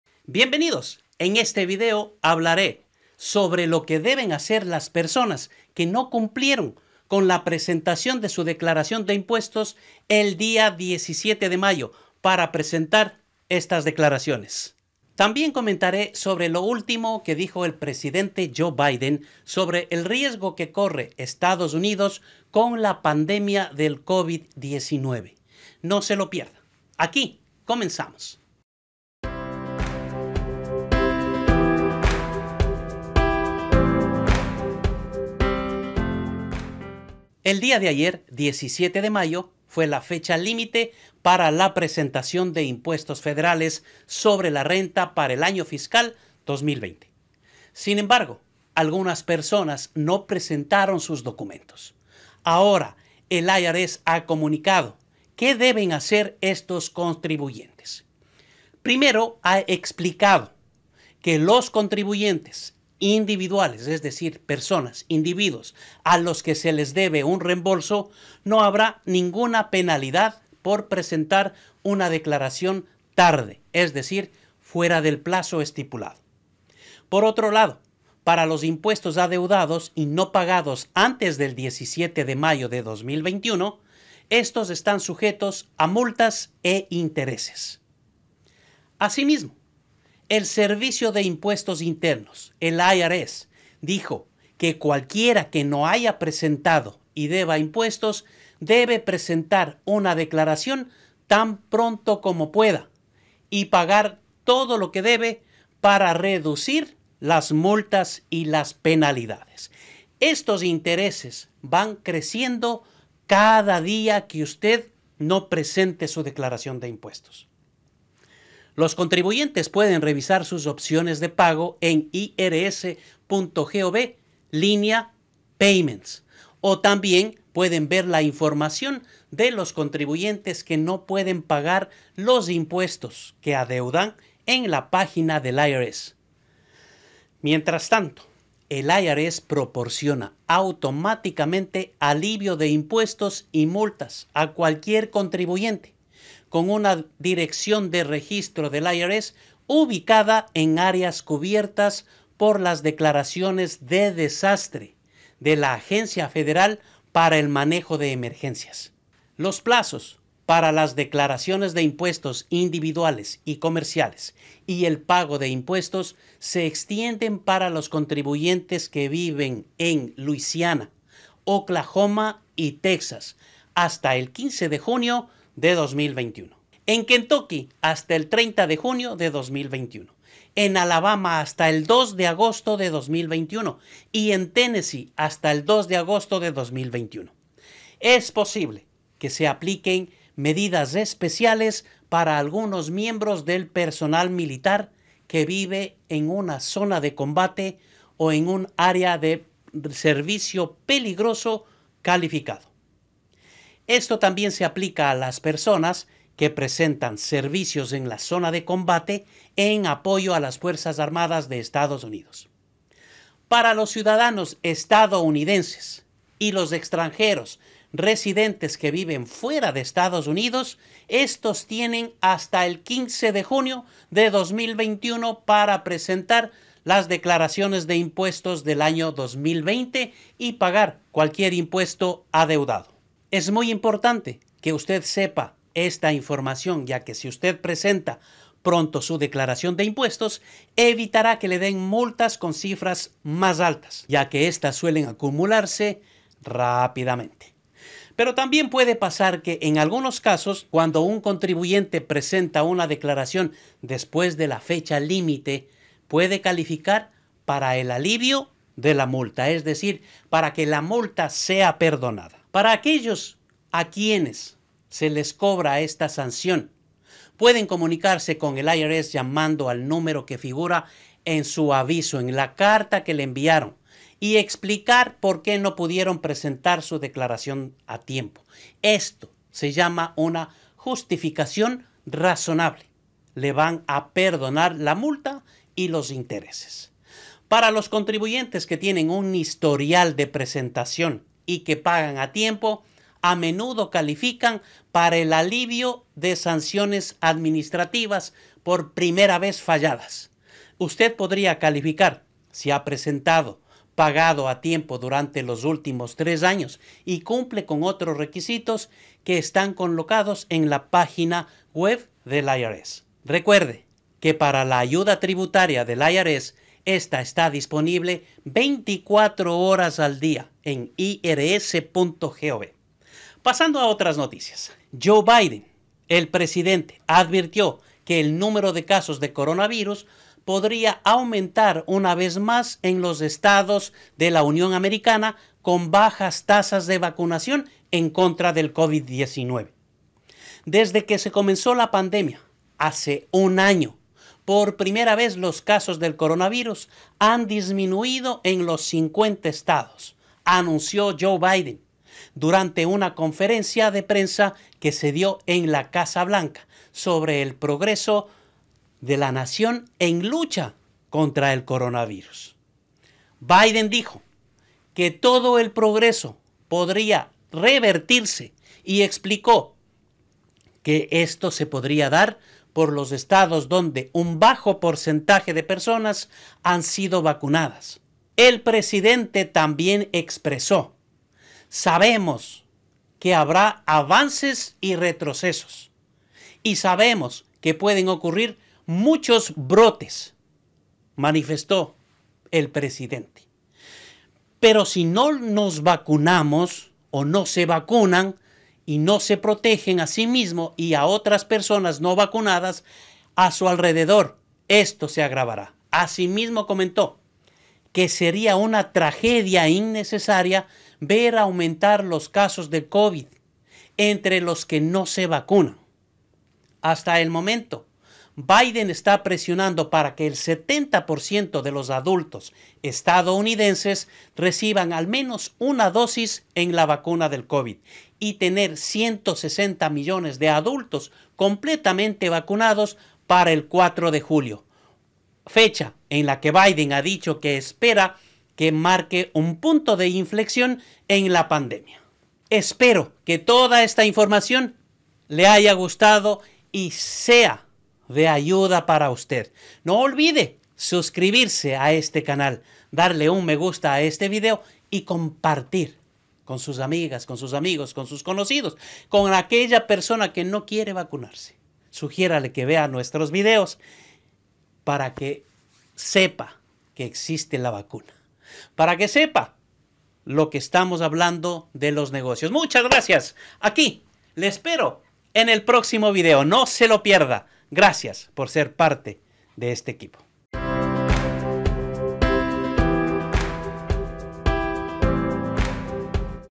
🔵El IRS explica qué deben hacer las personas que no cumplieron con la fecha límite del 17 de mayo
En este video hablaré sobre lo que deben hacer las personas que no cumplieron con la fecha límite del 17 de mayo para presentar sus impuestos. También comentaré sobre lo último que dijo el presidente Joe Biden sobre el riesgo que corre Estados Unidos con la pandemia del COVID-19. Si usted desea saber todos los detalles de estas noticias, no se despegue de su pantalla.

PÁGINA OFICIAL DE CARLOS RAMIREZ EN FACEBOOK